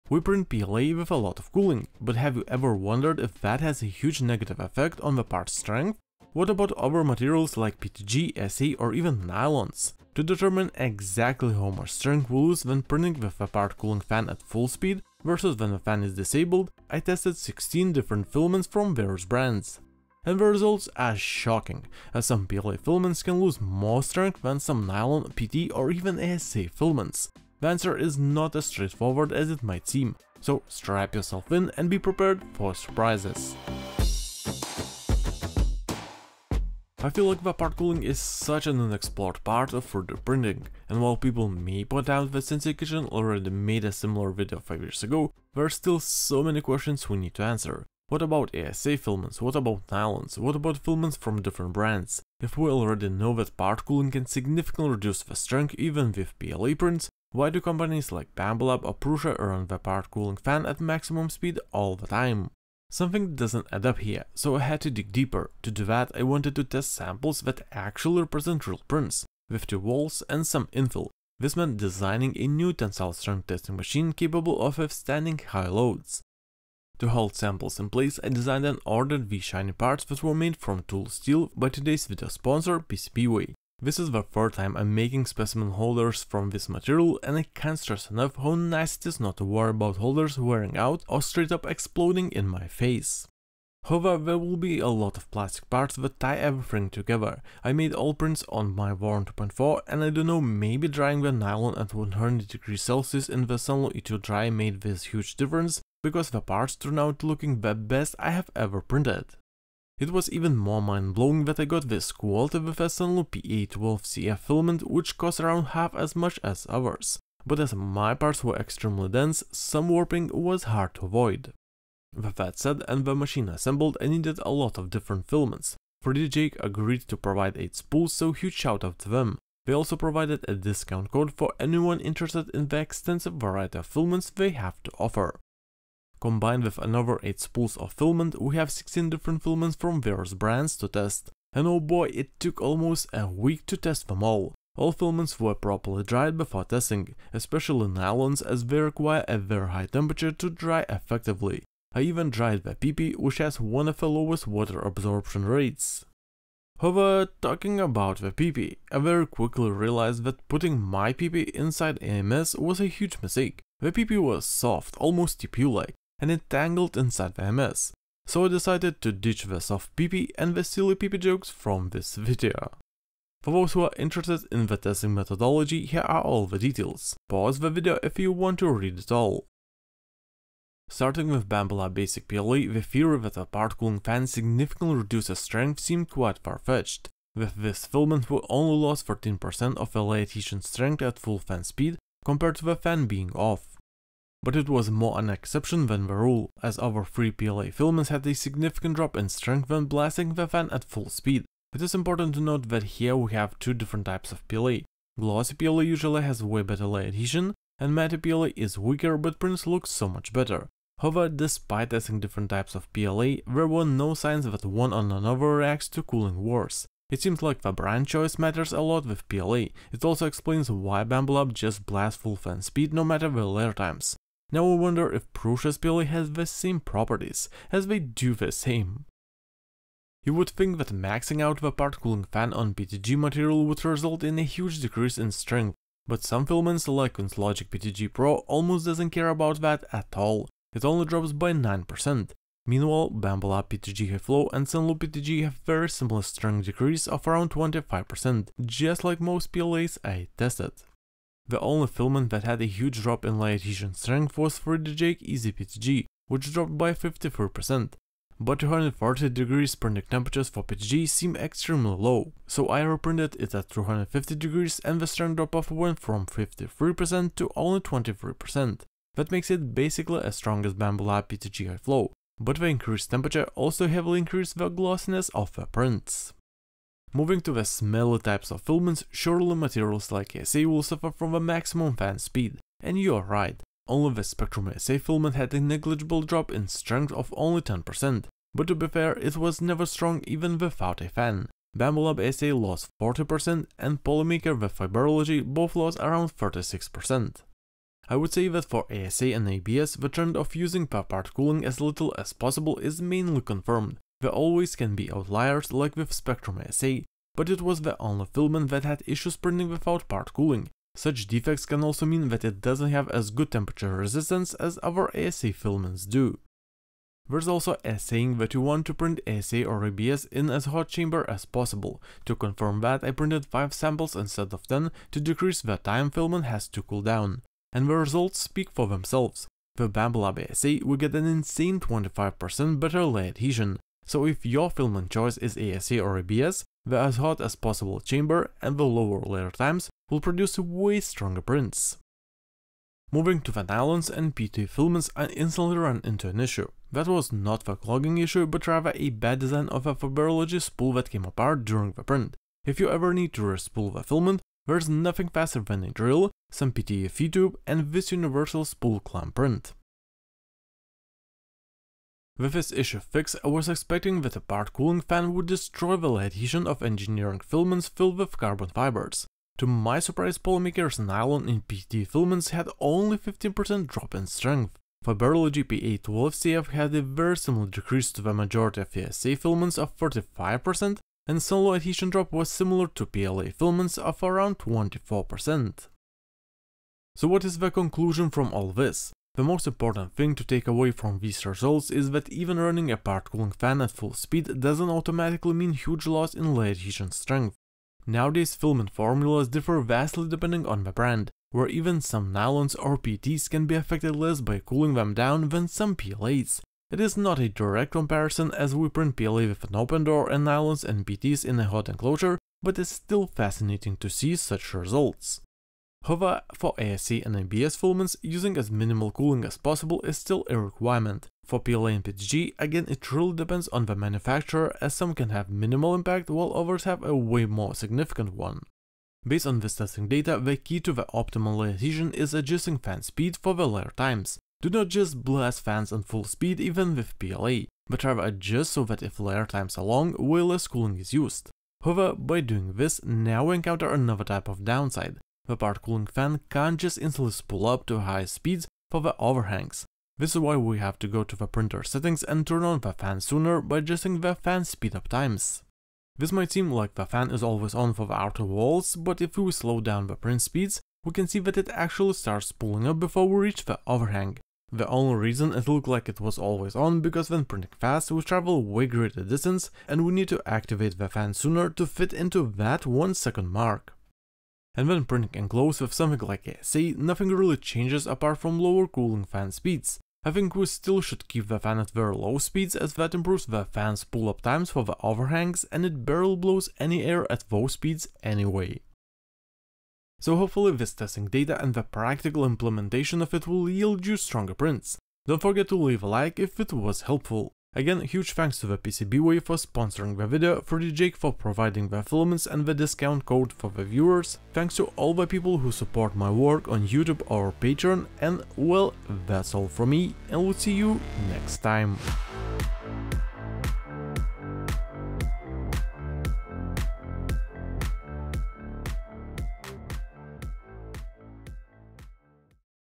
3D Printer PART COOLING Effects on LAYER ADHESION Strength (PLA, PETG, ASA, PA12, PET)
) for a wide selection of filaments – all in one spot!

👉🏻 In this video, "3D Printer PART COOLING Effects on LAYER ADHESION Strength", I am testing 16 different filaments to see how much the maximum cooling fan speed affects the layer adhesion strength of our prints versus disabling the fan. The results are shocking in both good and bad ways! But don't worry, I've got the solution!

3D printers:
- Phaetus Conch Plus
Filament Dryer:

🕗 TIMESTAMPS:
00:00 - The problem
01:04  - How I tested
03:00 - PLA
03:49 - PETG
04:36 - ASA
05:42 - PA12 and PET
06:27 - Summary and the solution